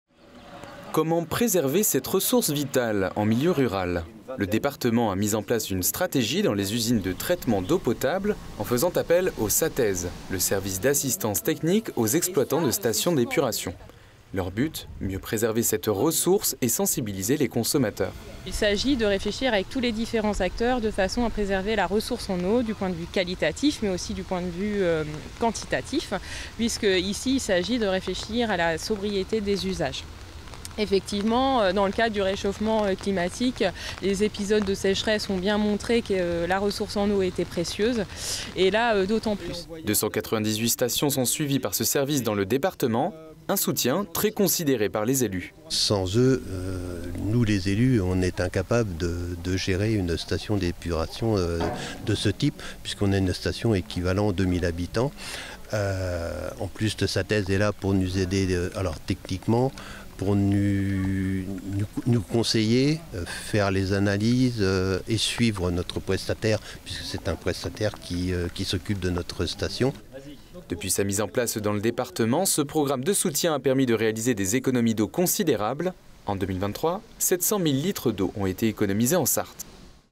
Station d'épuration : un plan départemental pour économiser l'eau
‎Comment préserver cette ressource vitale en milieu rural ?
Le département a mis en place une stratégie dans les usines de traitement d'eau potable, en faisant appel au SATESE : le Service d'assistance technique aux exploitants de station d'épuration.
Le cas à l'usine de traitement d'eau de Yvré-le-Pôlin.

Reportage : Alexandre Ferrere

📺 LMtv Sarthe sur votre télévision :
▸ BOUYGUES canal 369